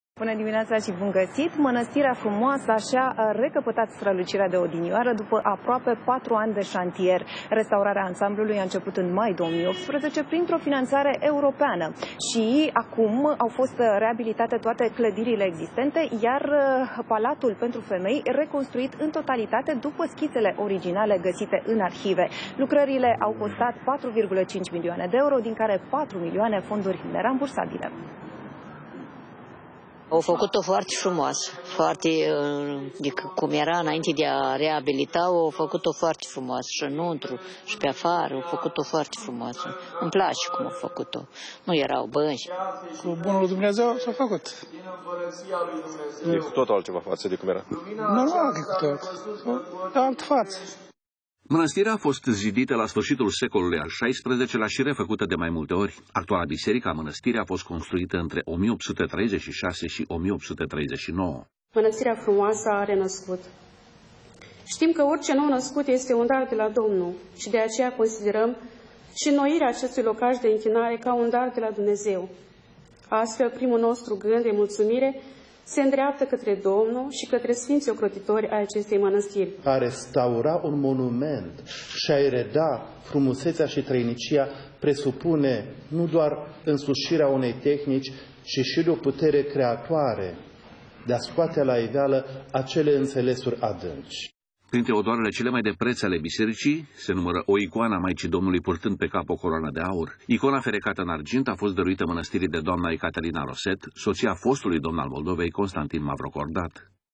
S-au finalizat lucrările de restaurare a Mănăstirii Frumoasa din Iași
O mănăstire din secolul al XVII lea a fost restaurată  cu fonduri europene. Mănăstirea Frumoasa şi-a recăpătat strălucirea de odinioară, după aproape 4 ani de şantier. Restaurarea ansamblului a început în mai 2018, printr-o finanțare europeană. Acum au fost reabilitate clădirile existente și a fost reconstruit în totalitate Palatul pentru femei, după schițele originale găsite în arhive. Lucrările au costat 4,5 milioane de euro, din care 4 milioane au fost fonduri nerambursabile.